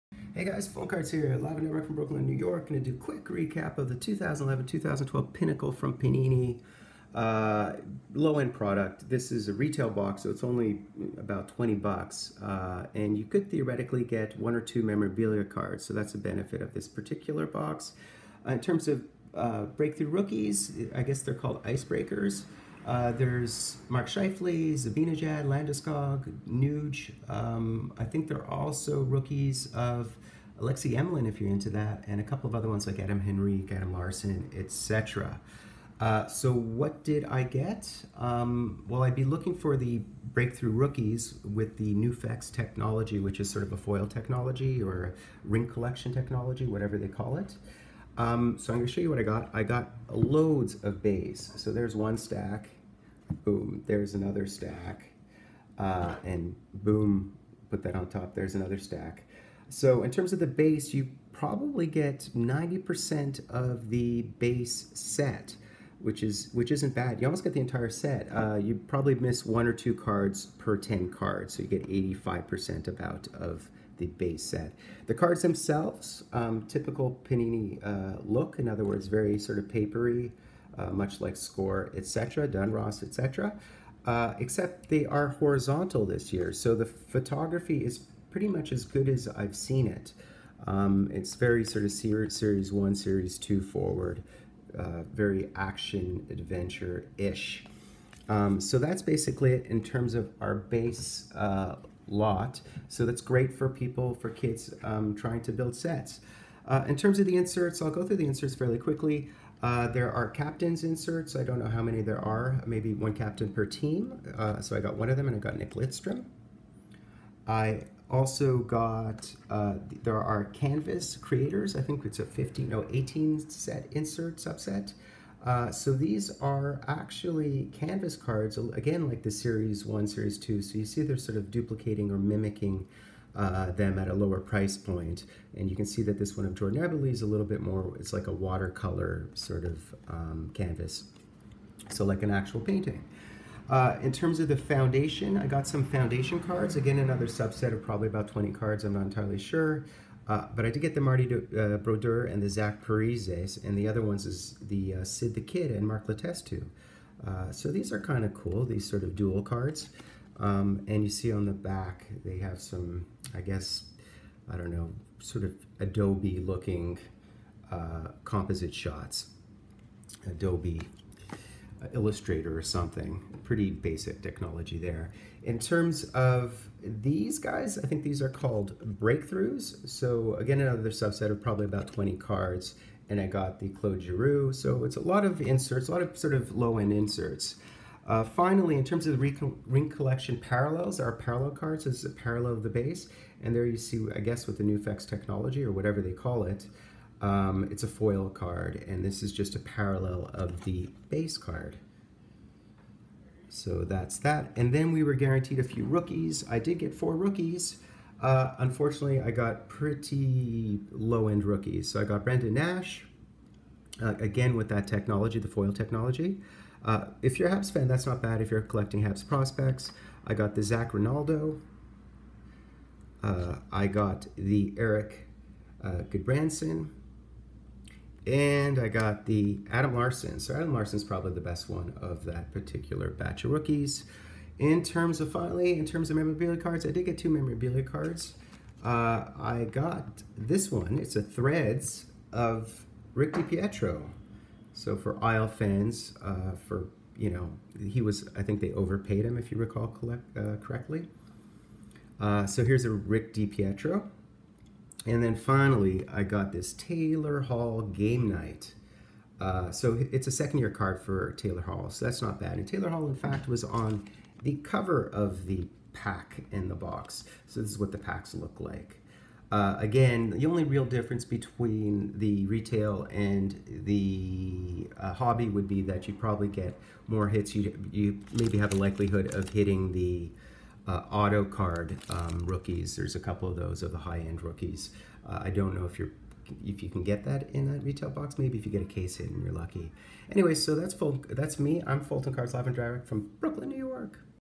2011-12 (11/12) Panini Pinnacle Hockey Retail Box Break | SPEEDY RECAP (2020)
2011-12 Panini Pinnacle Retail Box Recap and Review — by Fultoncards — Nugent-Hopkins?

I give away cards to those who help promote my channel.

Look for Ice Breakers Autographs, featuring the hottest rookies from the Class of 2011, including Ryan Nugent-Hopkins and Gabriel Landeskog.

Look for exclusive Foundation Tandems East inserts.

2011-12 Pinnacle Hockey features a 290-card base set including 250 commons, 30 Ice Breakers RCs and 10 Ice Breakers Autographs RCs.